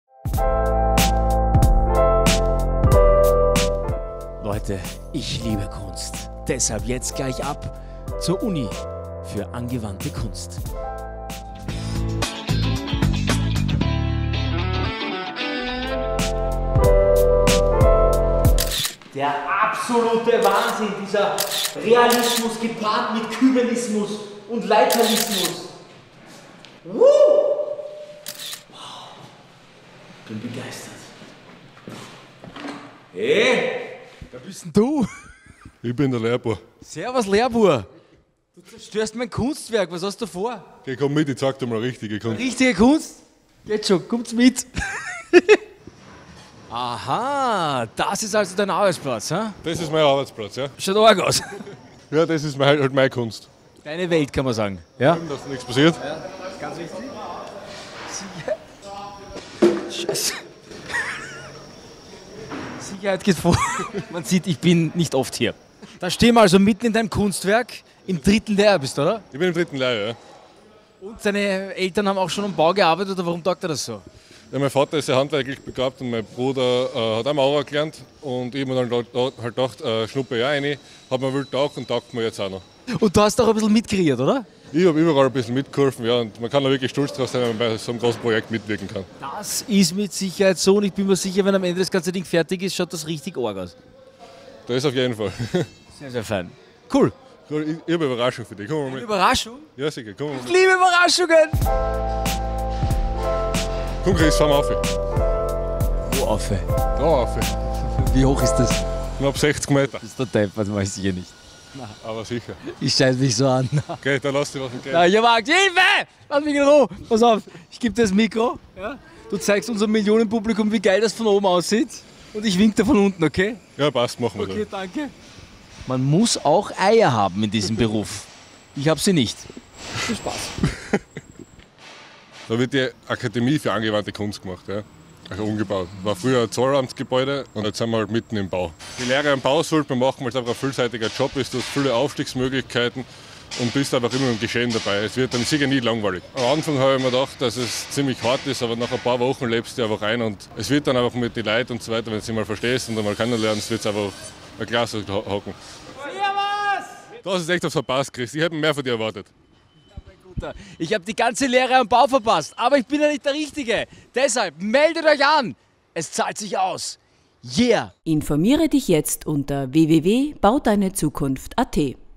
Zukunft bauen – Erfolgsgeschichten mit Chris Stephan – Teil 1
Baulehrlinge sind auch Künstler: Mit der Baulehre ist Karrieremachen keine Kunst!